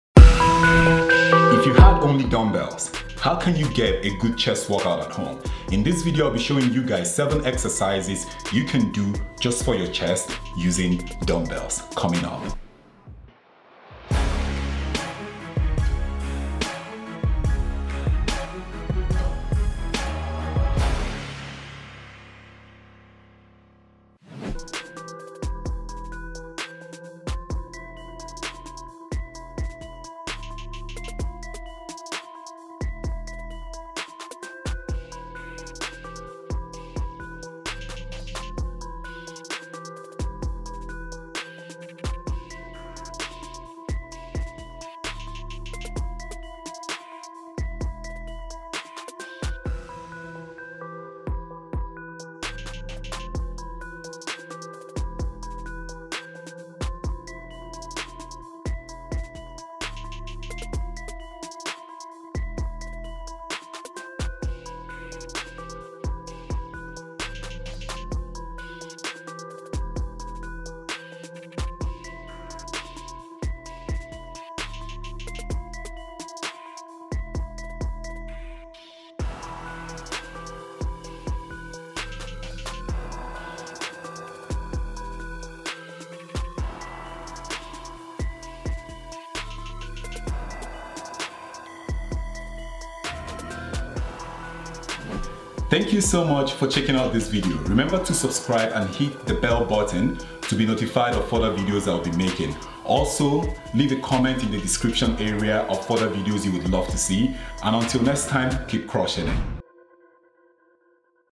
7 Chest Workout At Home With Dumbbells
7 Chest Workout At Home With Dumbbells.

Are you an individual who due to family, work and location can't get to the gym often, but still want to get fit at home? Then, you are in the right place.
In this video, I shared 7 chest workout at home with dumbbells (No Bench) to help you get started.
For best results, all my exercises are 10 sets of 10 repetitions. This would allow you to try each exercise once a day without getting bored easily.

To get started on your weight loss journey while building a lean muscle mass, visit my